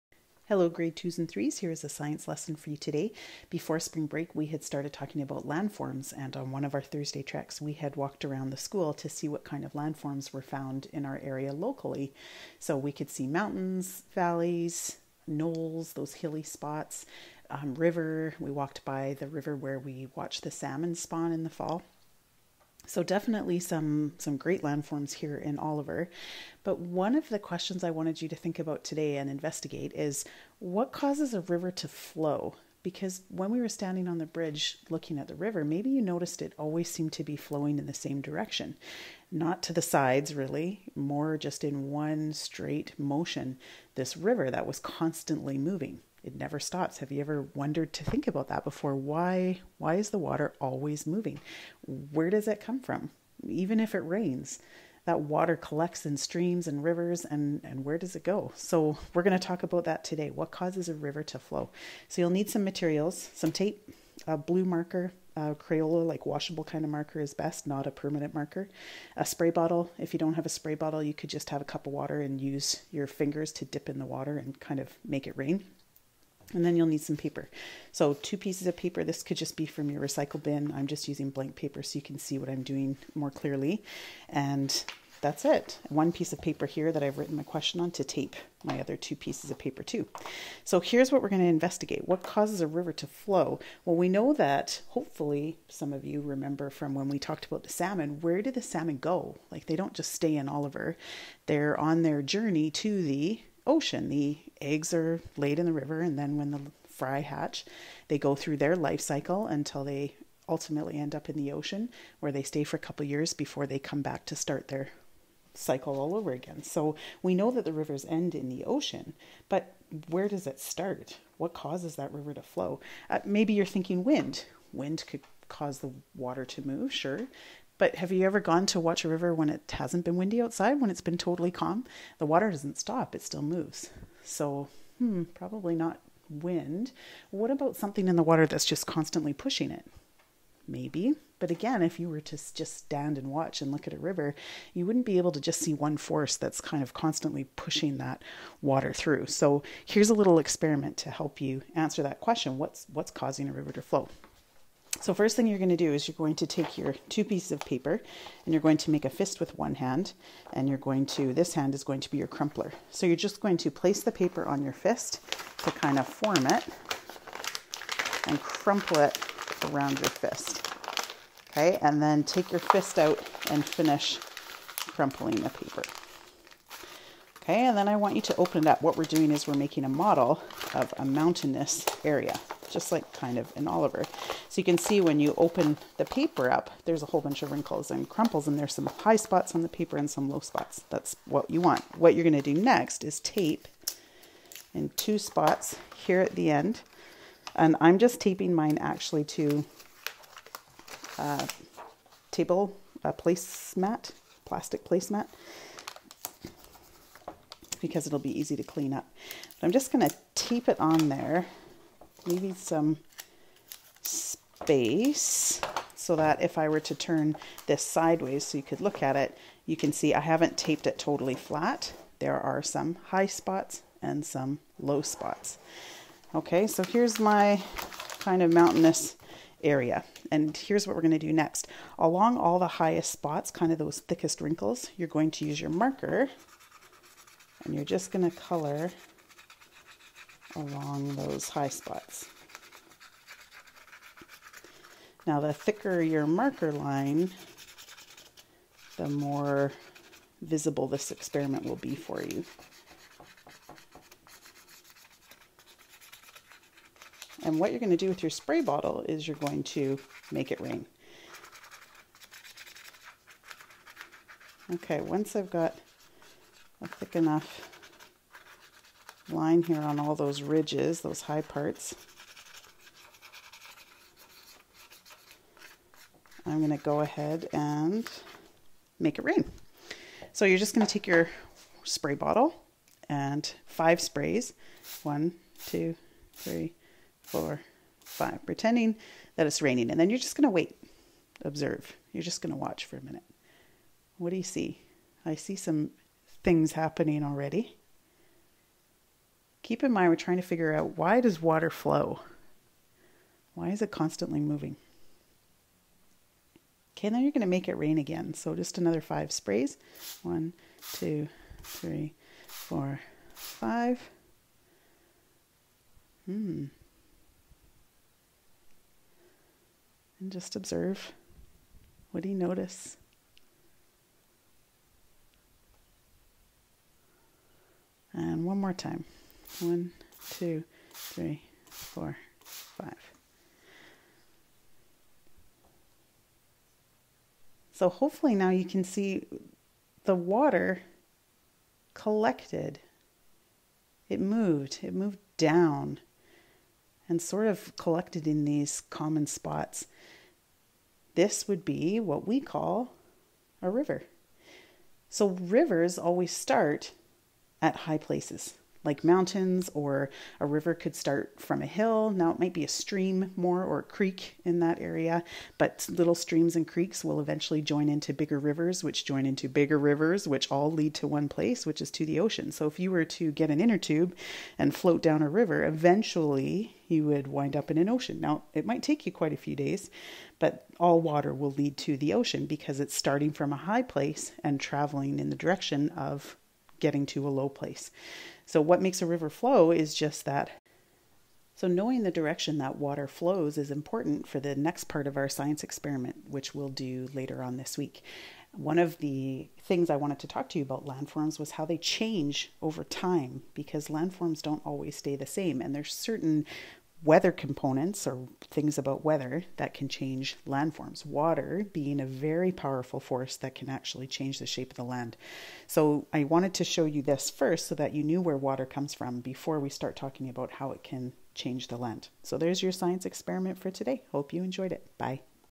April 14 - Science Inquiry - Why does water flow?
Have you ever wondered why water is always moving? Where does it go? What causes it to flow? All you need is some paper, tape, blue marker and a spray bottle of water in order to find out.
*for more see Mystery Doug